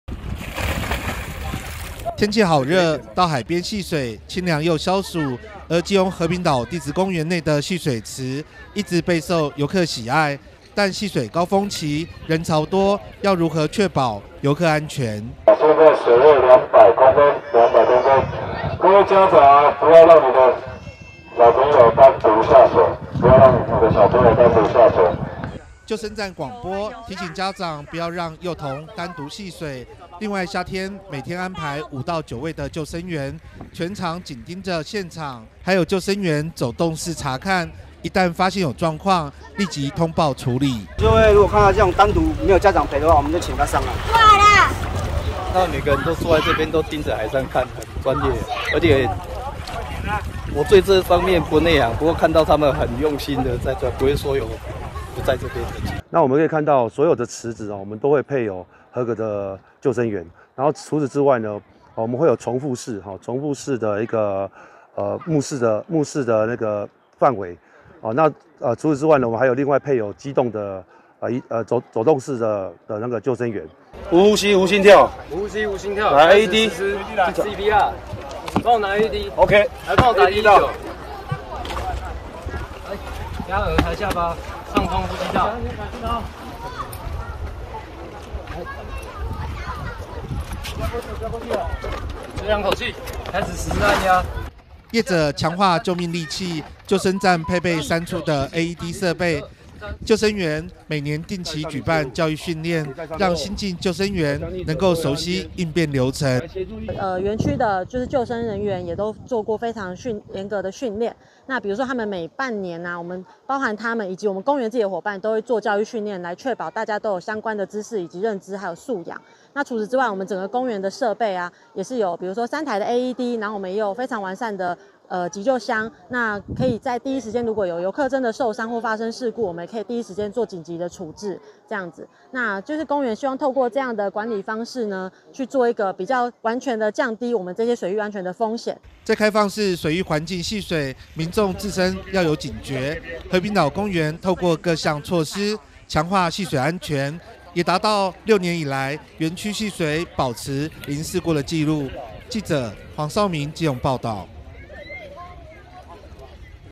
1120825 基隆 紹銘 和平島公園高規格救生標準 6年零事故
救生員｜教育訓練｜和平島公園高規格救生標準 6年零事故

(中嘉吉隆新聞 記者黃紹銘/基隆報導) 大武崙沙灘日前傳出溺水意外事故，凸顯夏天戲水安全重要性，而在基隆和平島地質公園，夏季戲水高峰，每天配置5-9名救生員，隨時注意每個角落，隨時提醒家長避免讓幼童孩子單獨戲水，每年固定進行救生員教育訓練，希望確保泳客安全。
天氣炎熱，民眾到海邊泡水，清涼又消暑，而基隆和平島地質公園內的戲水池，一直以來備受遊客喜愛，但戲水高峰期，人潮多，要如何確保遊客安全。
救生站廣播，提醒家長不要讓幼童落單戲水，另外，夏季每天安排5-9位救生員，全程緊盯著現場，還有救生員走動式察看，一發現有狀況，立即通報處理。
業者強化救命利器，救生站配置3處AED設備，救生員每年定期舉辦教育訓練，讓新進救生員，熟悉應變流程。
在開放式水域環境戲水，民眾自身要有警覺，和平島公園業者透過各種措施，強化戲水安全，也達到6年來園區戲水保持零事故的紀錄。